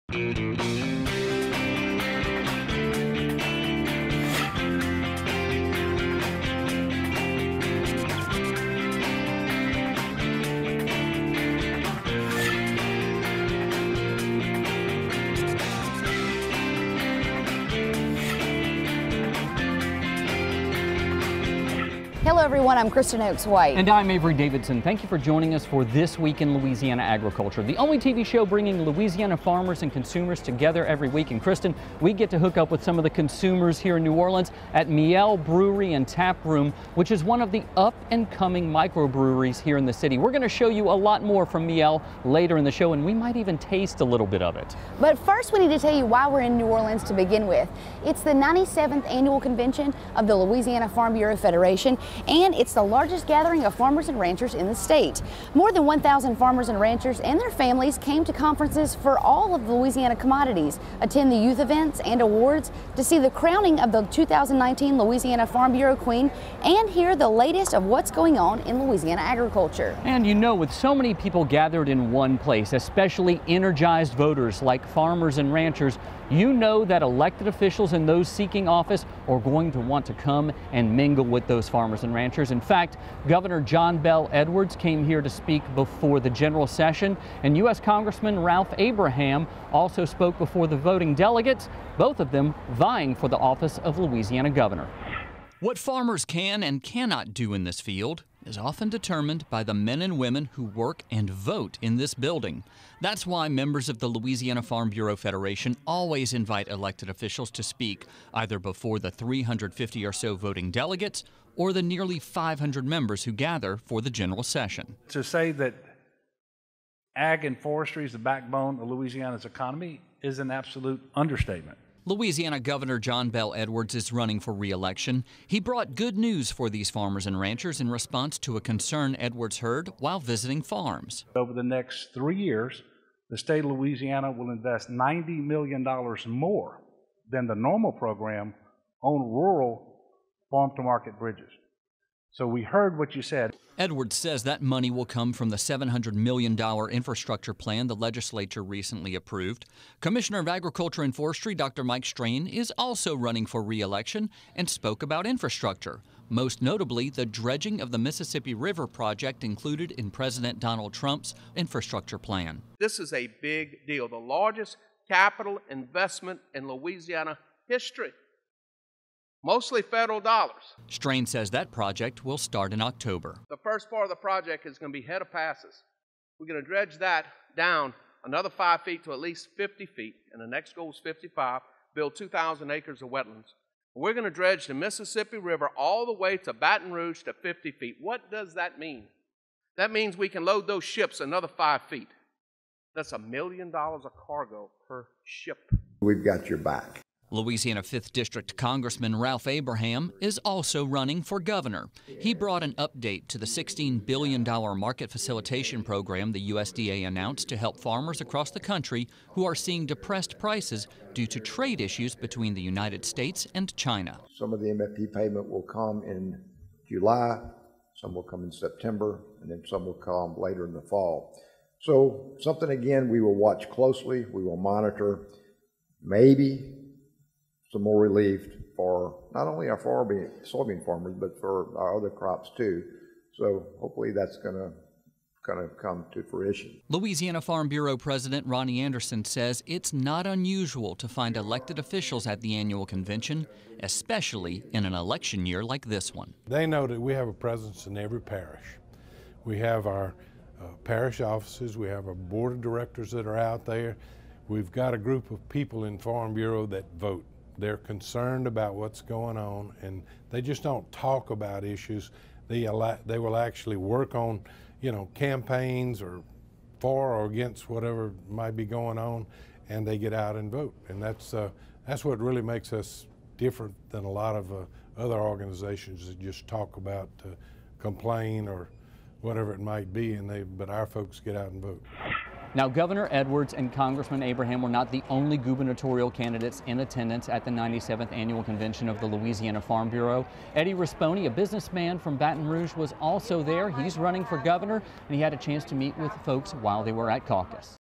Seeking Your Vote
Last week, more than 1,000 farmers and ranchers and their families traveled to the New Orleans Marriott for the 97th Annual @Louisiana Farm Bureau Federation Convention. With so many people in one place, especially when those people are energized voters like farmers and ranchers, you know those seeking office are going to come here, too. That's one of the reasons why Governor John Bel Edwards, who is seeking reelection, and Congressman Ralph Abraham, who wants Edwards' job, came to speak to members of the Louisiana Farm Bureau.